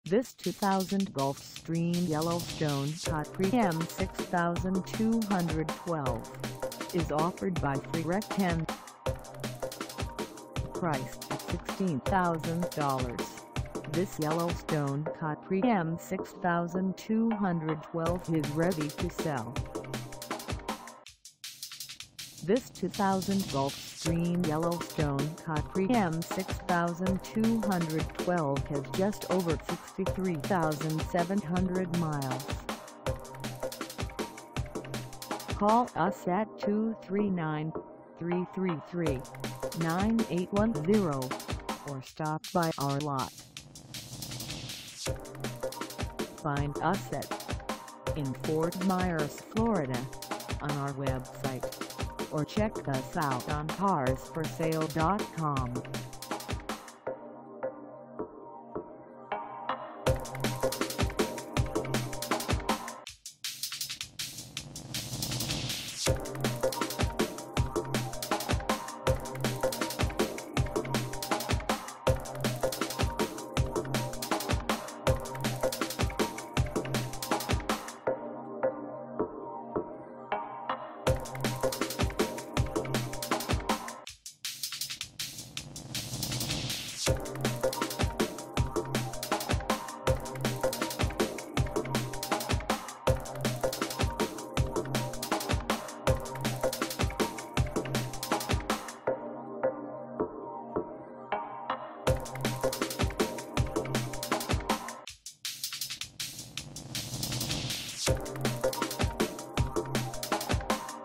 2000 GULF STREAM YELLOWSTONE CAPRI M-6212 - for sale in For
FreeRek 10

Come test drive this 2000 GULF STREAM YELLOWSTONE CAPRI M-6212  for sale in Fort Myers, FL.

2000 YELLOWSTONE CAPRI BY GULF STREAM, CLASS C MODEL M-6212, 21' WITH 2 SLIDE OUT. FORD E-450 CHASSIS AND POWERED BY FORD TRITON V-10, SLEEPS 6 GUESTS, SOFA WITH HIDE A BED, OVERHEAD BED. ONAN 4KW GENERATOR, POWER STEP, FULL BATH WITH TUB AND SHOWER, , REAR KITCHEN WHICH INCLUDES, DOUBLE DOOR REFRIDGERATOR/FREEZER, OVEN, 3 BURNER RANGE, MICROWAVE. RECENTLY HAD SERVICE PERFORMED- OIL CHANGE, TIRES BALANCED, NEW BRAKE PADS, NEW CALIPERS AND NEW ROTORS.